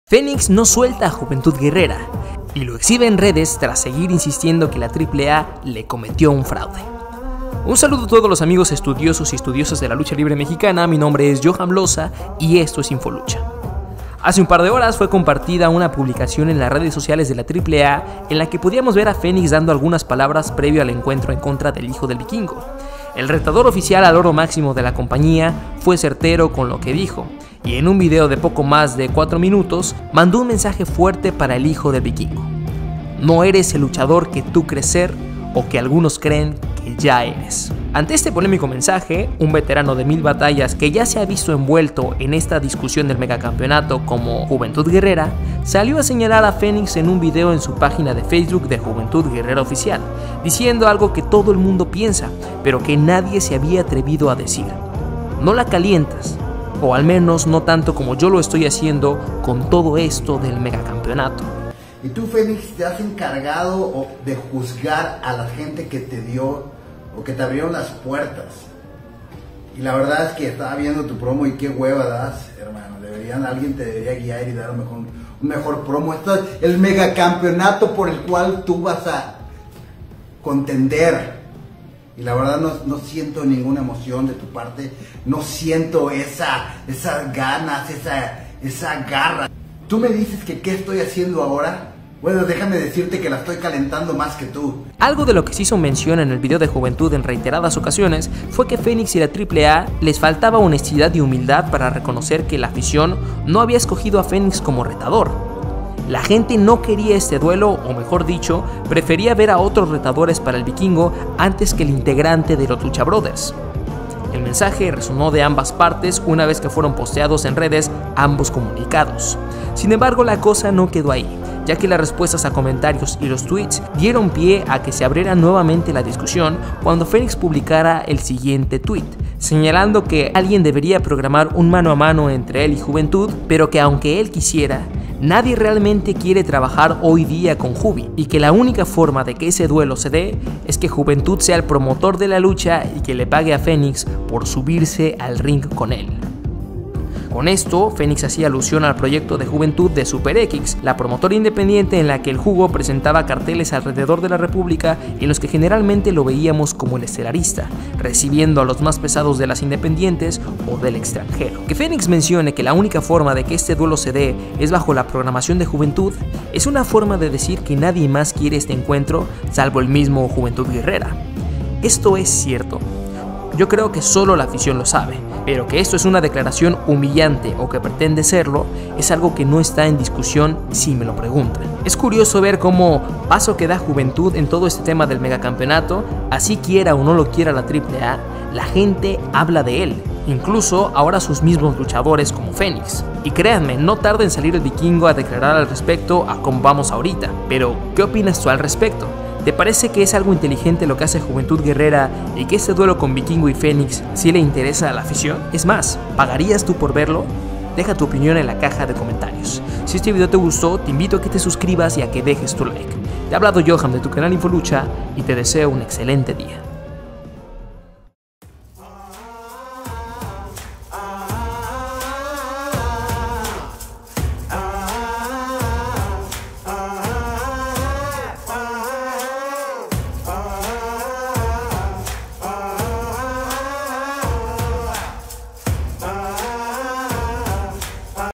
Fenix HUNDE a JUVENTUD GUERRERA y lo EXHIBE como Promotor
Después del anuncio del retador oficial de Megacampeonato del Triplemania 30, la inconformidad de Juvi se ha hecho notar en casi todas las ocasiones. Pero hoy las cosas fueron un poco más alla, con la respuesta de Fénix que dejó helado al "Jugo de la lucha libre"

Si quieres ir aún más allá, dale a la campanita.